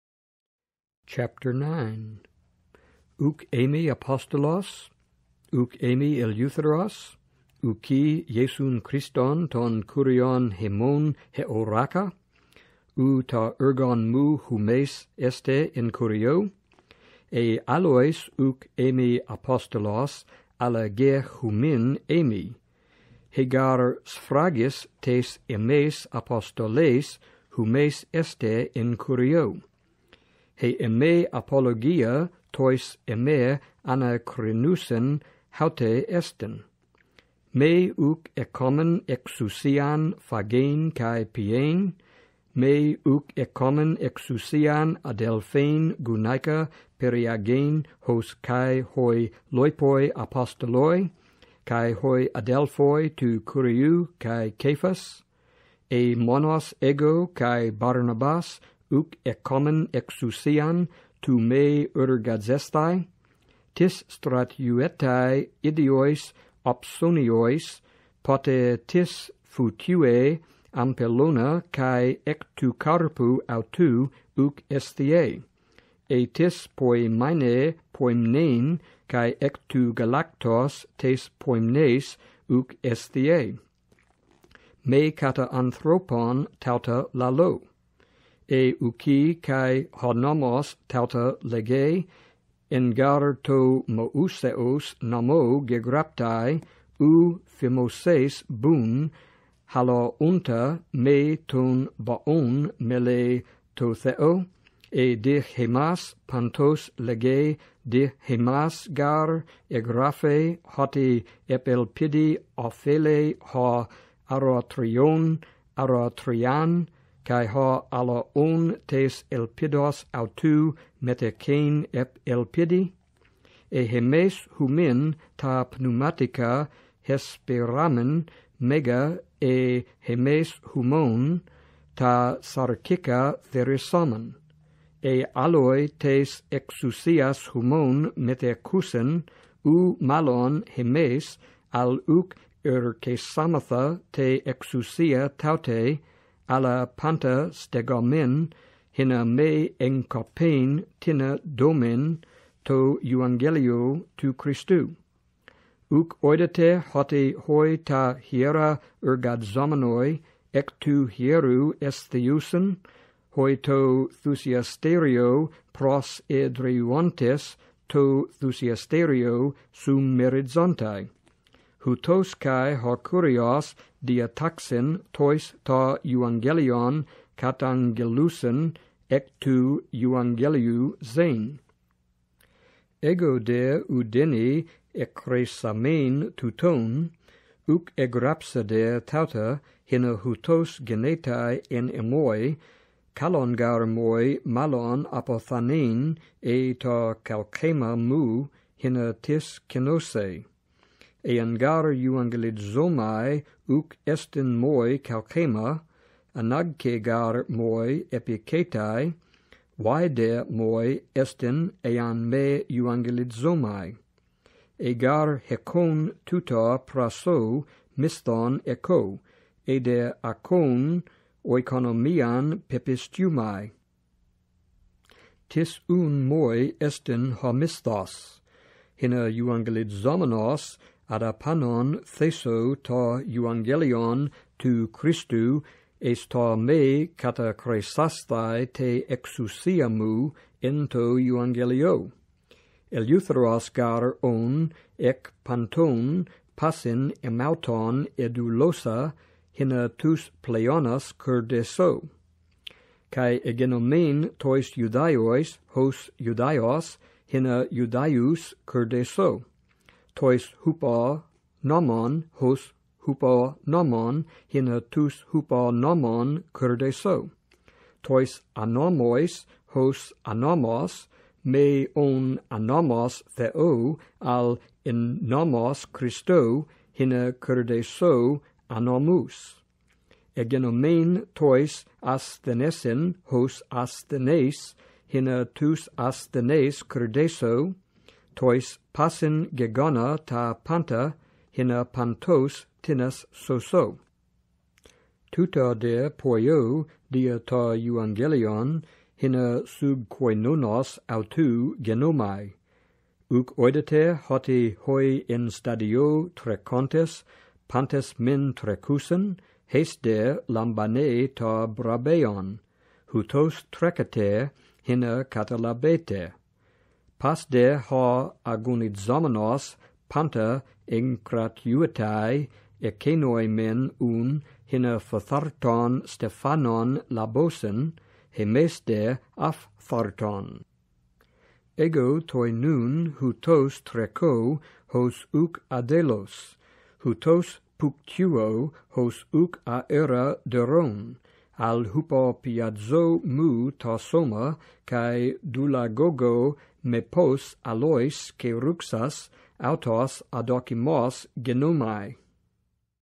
I Corinthians 9 in Greek (Maurice Robinson)
Maurice Robinson reeds I Corinthians 9 in Greek (Robinson-Pierpont 2005 edition).